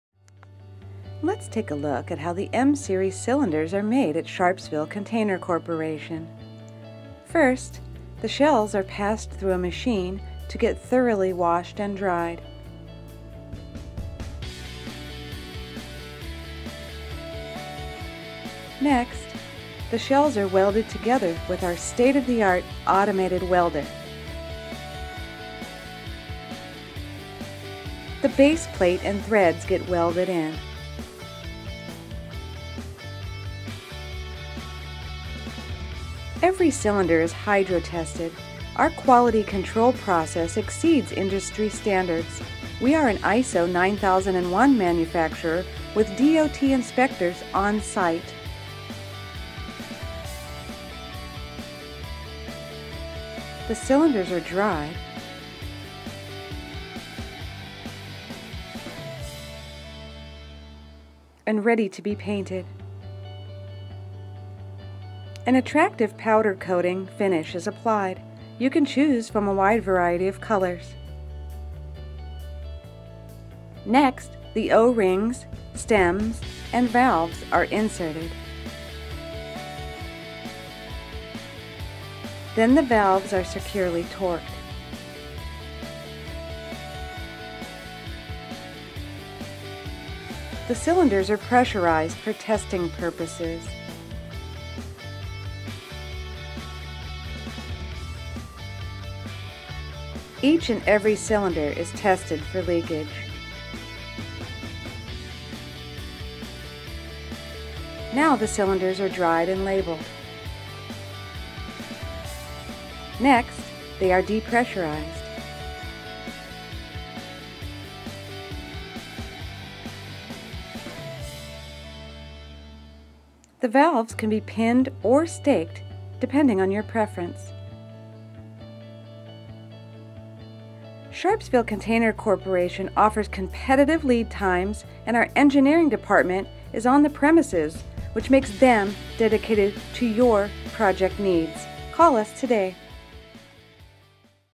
How They're Made - Sharpsville Container M Series Cylinders
This is a video of how Sharpsville Container's M Series high pressure cylinders are made.